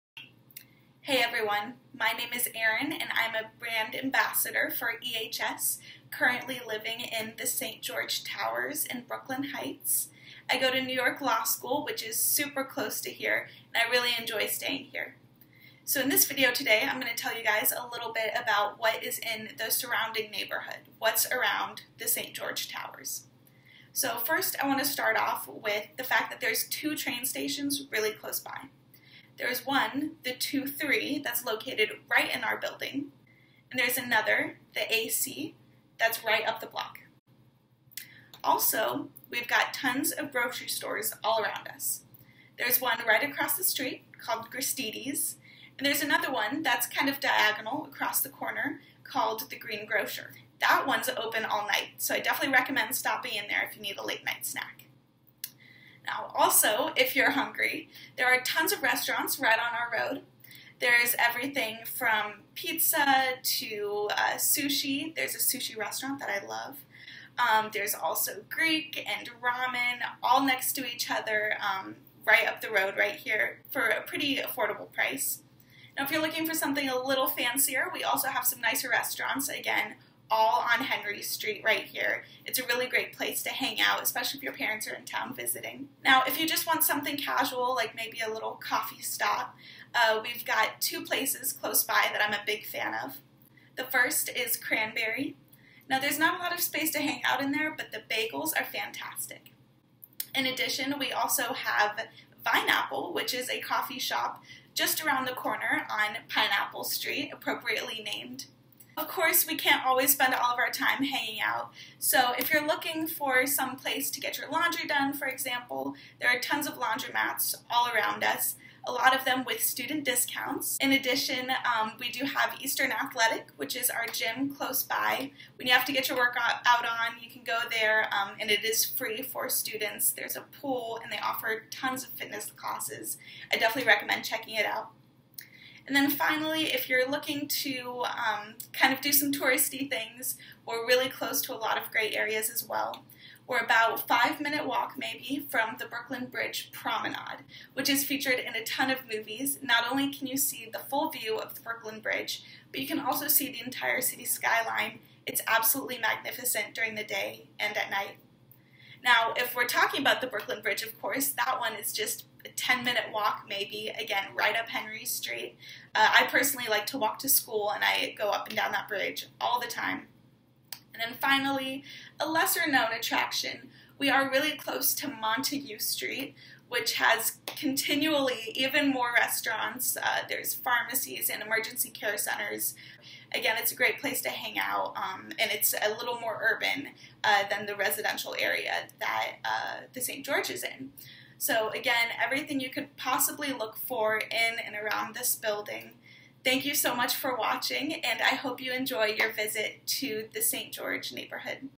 Tour the EHS St. George Towers Neighborhood - Brooklyn Heights
EHS St. George Towers resident Erin shares her favorite spots in the beautiful Brooklyn Heights neighborhood. or (Please note: The St. George Towers does not feature a gym amenity at this time. Please check the official building page for the full list of amenities by building.)